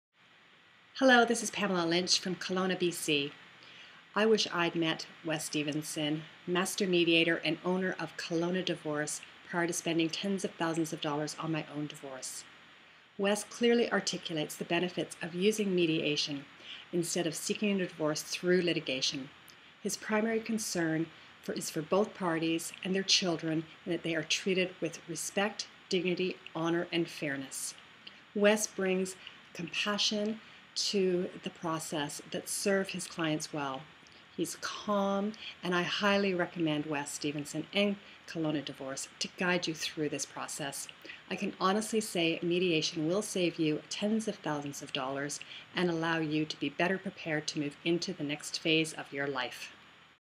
Kelowna Divorce, Family and Divorce Mediator, Wes Stevenson
Do you need to know how to get a divorce, and be guided through the process in at a cost much lower than litigation? Wes Stevenson, of Kelowna Divorce, is a family and child support mediator, a facilitator and family law specialist.